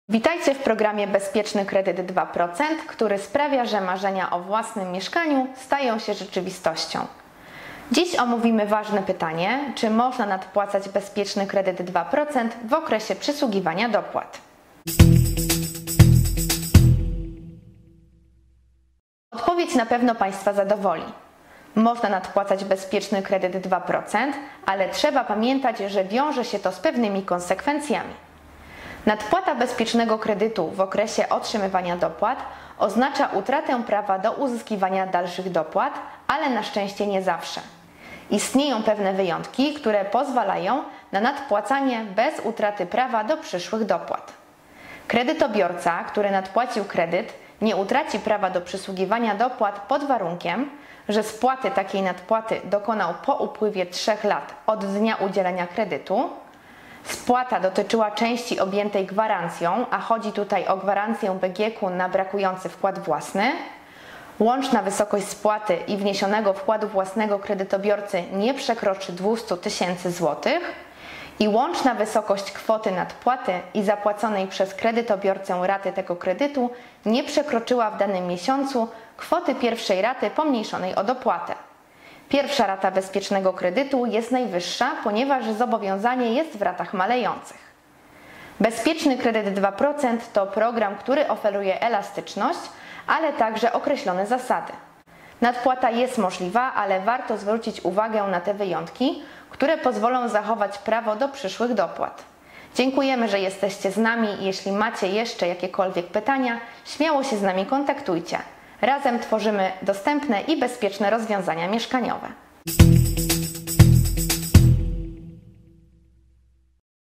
Kiedy warto nadpłacać Bezpieczny Kredyt 2%? Poznaj zasady!
📢 Drodzy Beneficjenci Programu Bezpieczny Kredyt 2% - im więcej wiecie na temat zasad Programu, tym lepiej z niego korzystacie 🧠💡
Zastanawialiście się, czy można nadpłacać Bezpieczny Kredyt 2% w okresie przysługiwania dopłat rządowych? 🤔💰

Tak! Należy mieć jednak na uwadze wynikające z tego konsekwencje - w pewnych sytuacjach może się to wiązać z utratą prawa do otrzymywania dalszych dopłat 🚫
Pamiętaj, że jeśli jesteś beneficjentem jakiegoś programu, to bardzo ważne jest zdobycie kompleksowej wiedzy na jego temat 📚🤓
Zapoznaj się z przypadkami, kiedy nie tracisz prawa do dopłat rządowych w ramach kredytu 2%:

✅dokonujesz nadpłat kredytu po 3 latach od podpisania umowy
✅spłata dotyczyła części objętej gwarancją - tu mowa o gwarancji BGK na brakujący wkład własny
✅łączna wysokość spłaty i wniesionego wkładu własnego kredytobiorcy nie przekroczy 200 000 zł
✅łączna wysokość kwoty nadpłaty i zapłaconej przez kredytobiorcę raty tego kredytu nie przekroczyła w danym miesiącu kwoty pierwszej raty pomniejszonej o dopłatę - Bezpieczny Kredyt 2% udzielany jest w ratach malejących, dlatego pierwsza rata jest najwyższa.

Im więcej wiesz, tym lepiej możesz czerpać z zalet dofinansowania - pamiętaj aby mądrze zarządzać finansami i zwracać uwagę na obowiązujące zasady 🤝📊